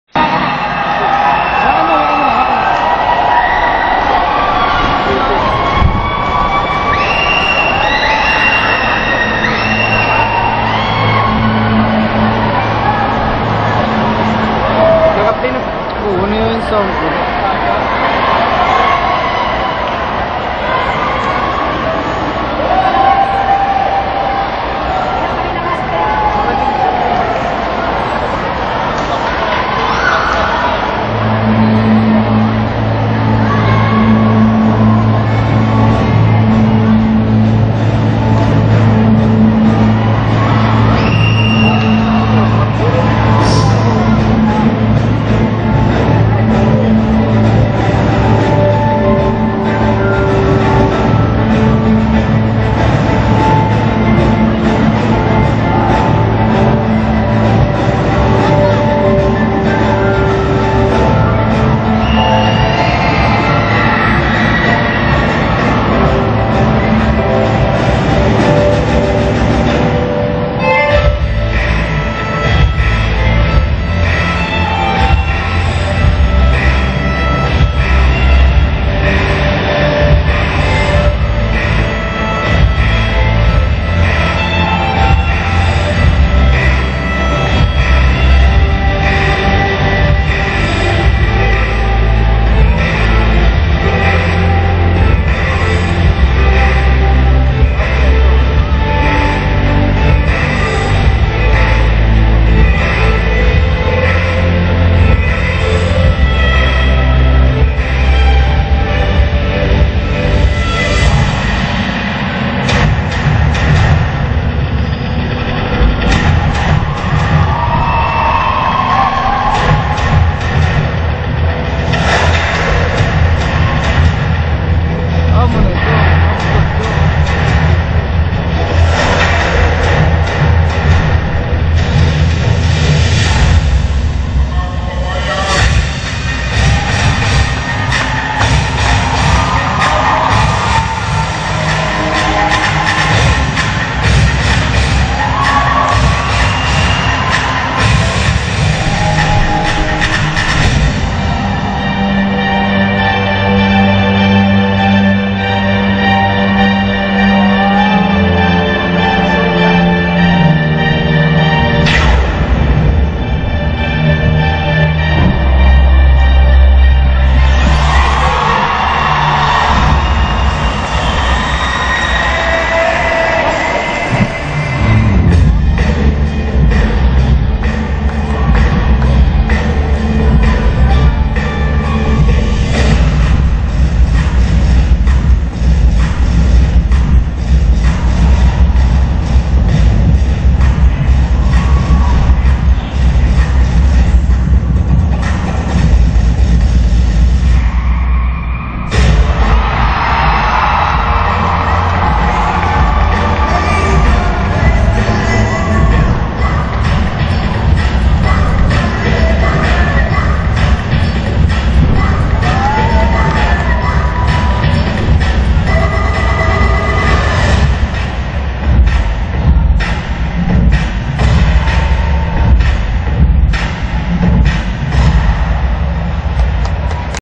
Philippine Island Assassin @ Fabulous Five 2011 Tacloban Astrodome
june 23 2011 performance @ Fabulous five tacloban astrodome, Tacloban City